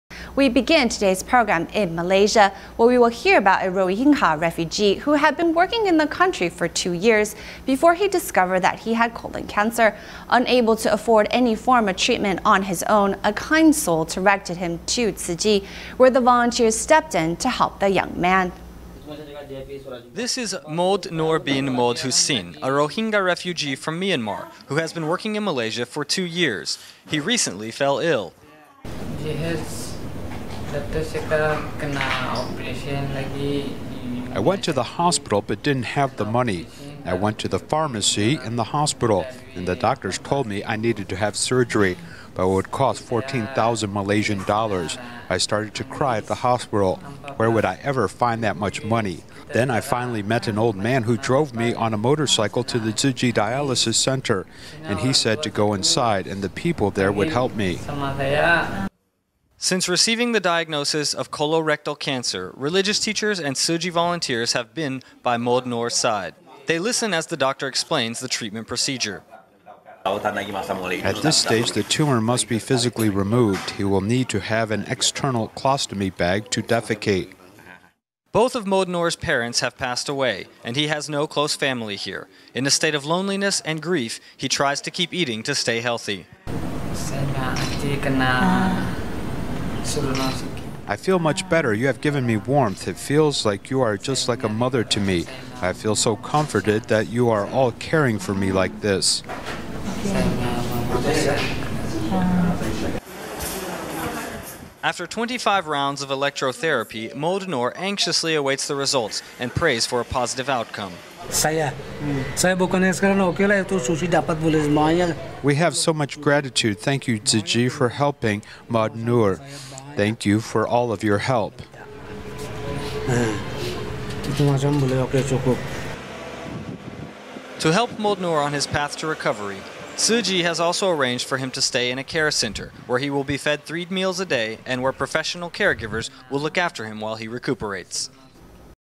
20161130 Tzu Chi comes to the aid of a refugee
We begin today’s program in Malaysia, where we will hear about a Rohingya refugee who had been working in the country for two years before he discovered that he had colon cancer. Unable to afford any form of treatment on his own, a kind soul directed him to the Tzu Chi, where volunteers stepped in to help the young man.